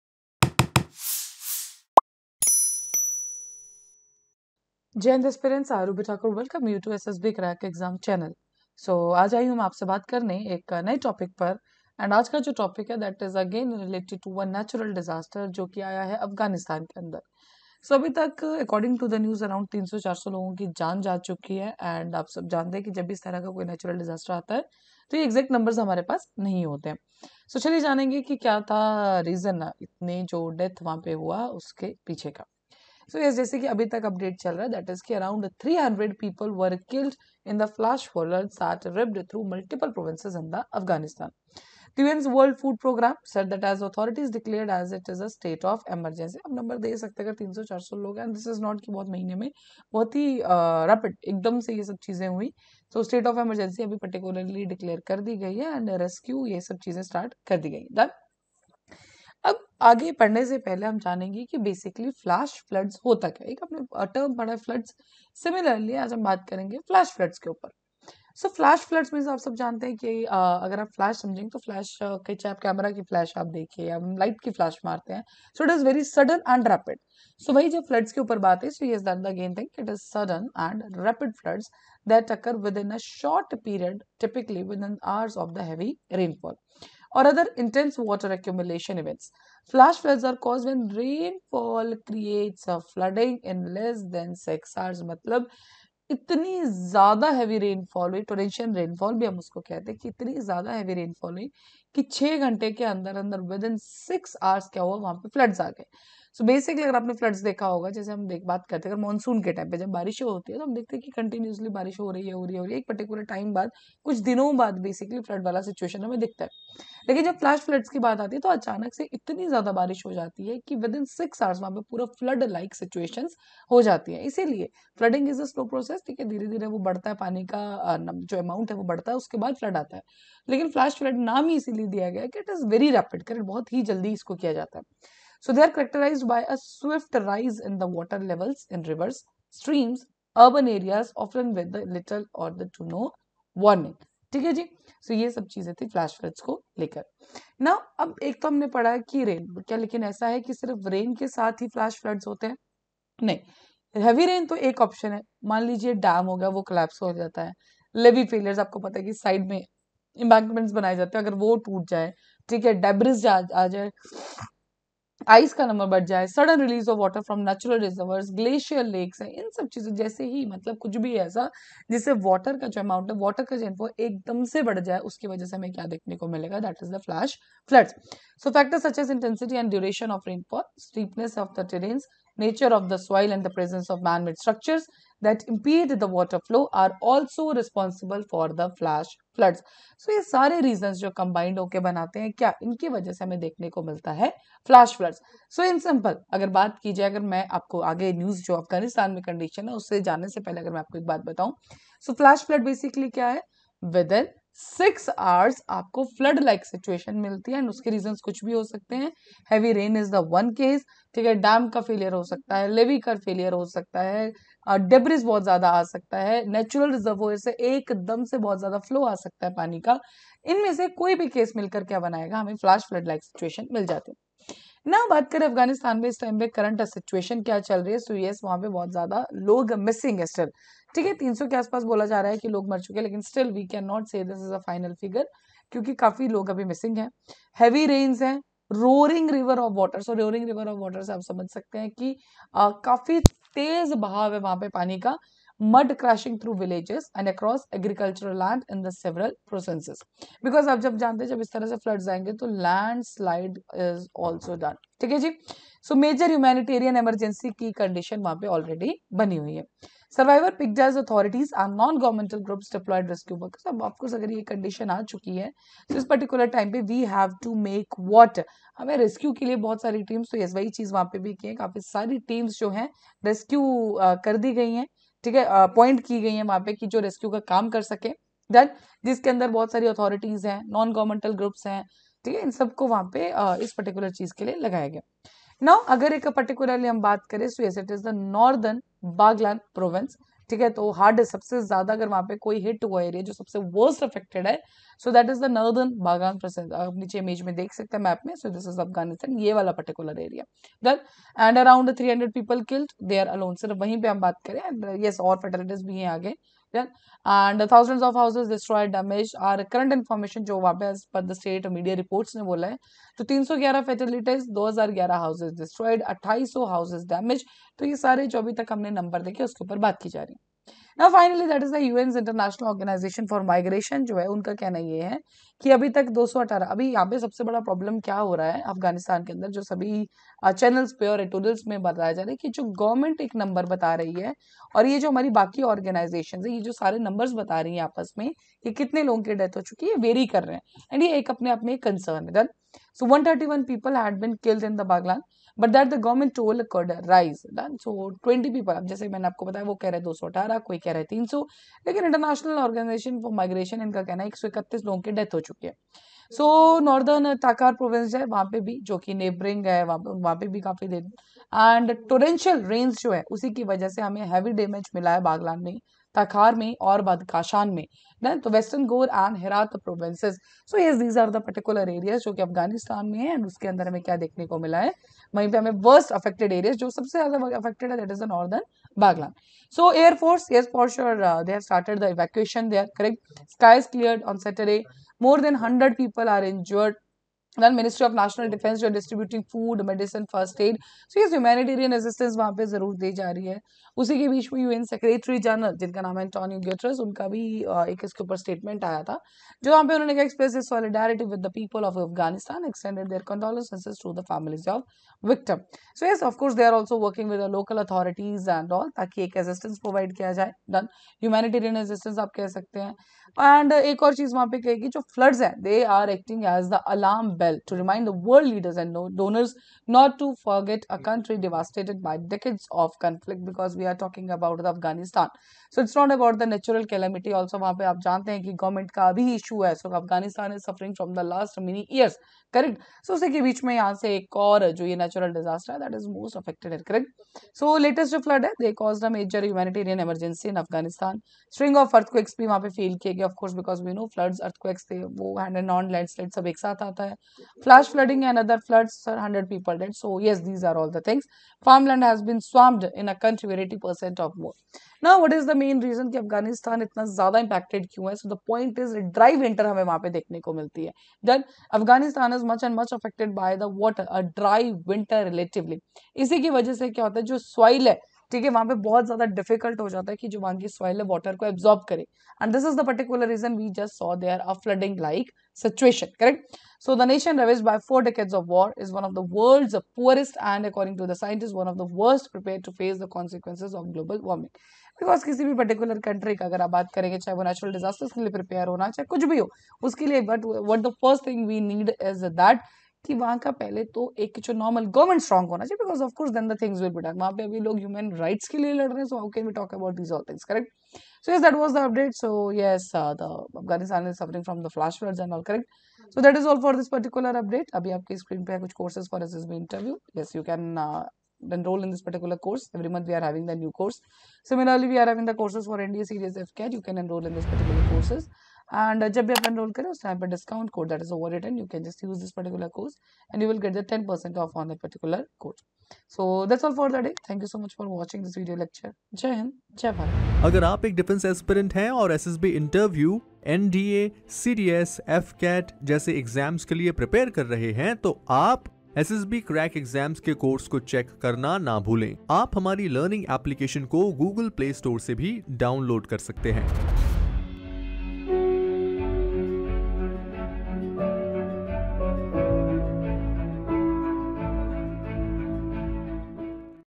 Afghanistan Flash Floods | UPSC | SSB Interview
More than 300 people were killed in flash floods that ripped through multiple provinces in Afghanistan, the UN’s World Food Programme said, as authorities declared a state of emergency and rushed to rescue the injured.